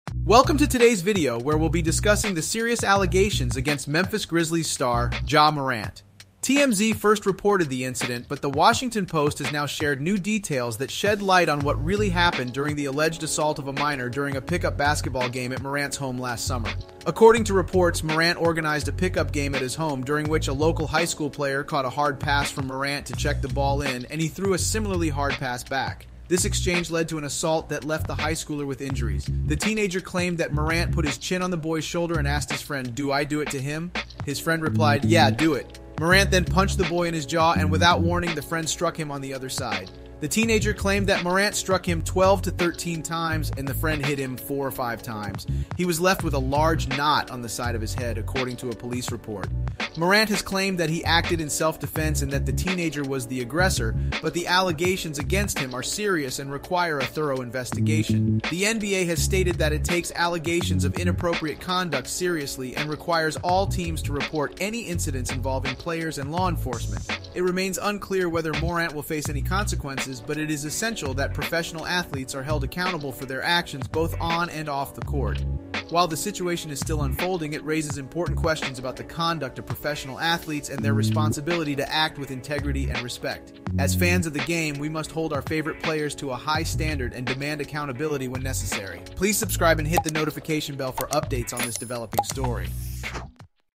Breaking News: NBA Star Ja Morant Accused of Assault During Pickup Game at Home
Welcome to today's video, where we'll be discussing the serious allegations against Memphis Grizzlies' star, Ja Morant. TMZ first reported the incident, but The Washington Post has now shared new details that shed light on what really happened during the alleged assault of a minor during a pickup basketball game at Morant's home last summer. According to reports, Morant organized a pickup game at his home, during which a local high school player caught a hard pass from Morant to check the ball in, and he threw a similarly hard pass back. Please continue watching to learn the full detials.